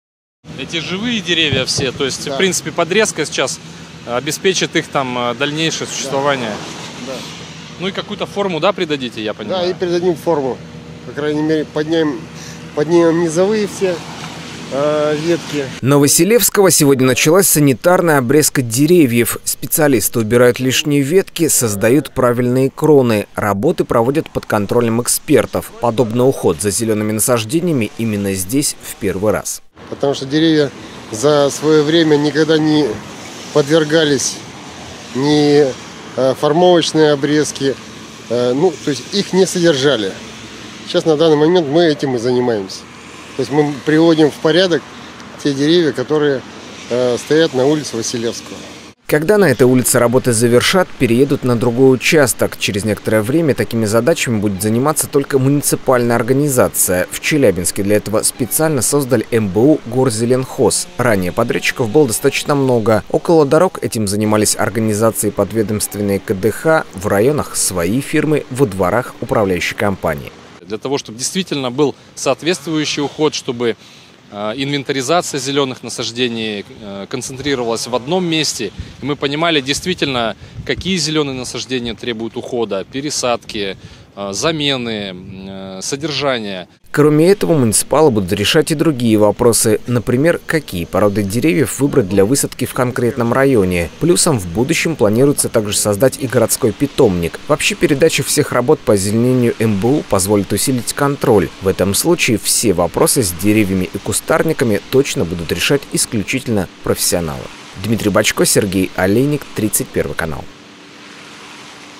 Озеленением будут заниматься муниципалы. В Челябинске создали МБУ «Горзеленхоз»
Через некоторое время следить за деревьями, кустарниками и газонами будет именно муниципальное предприятие. Сейчас эти работы проводят самые разные подрядчики. Почему в мэрии решили так сделать?